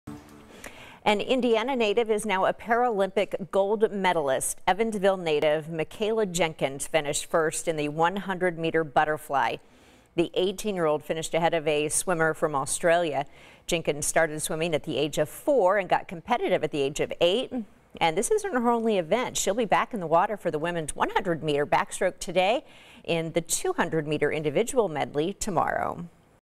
Evansville native Mikaela Jenkins wins gold at Paralympics
The 18-year-old finished first in the 100-meter butterfly - S10 swimming event at the Tokyo Paralympics.